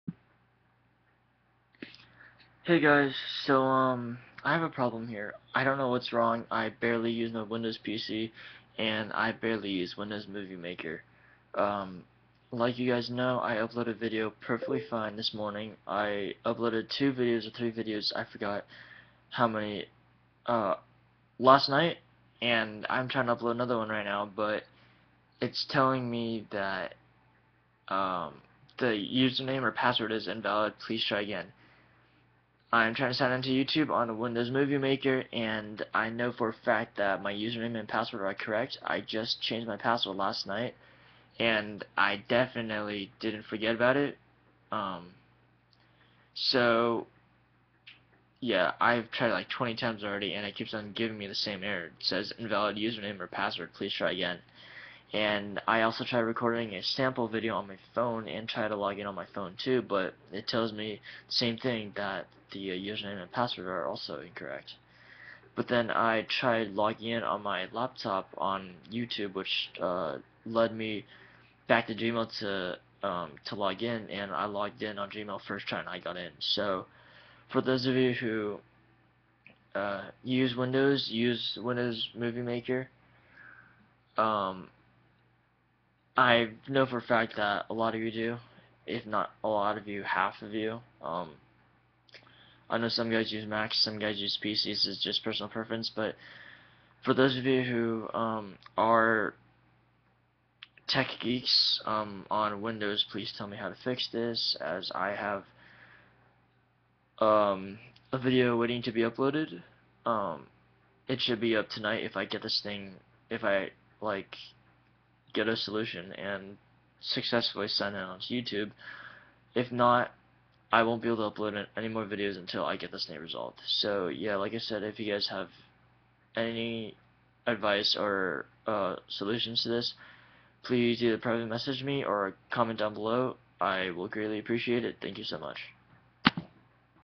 problem with windows movie maker
so basically everything i said in the video. gives me the 'invalid username or password. please try again.'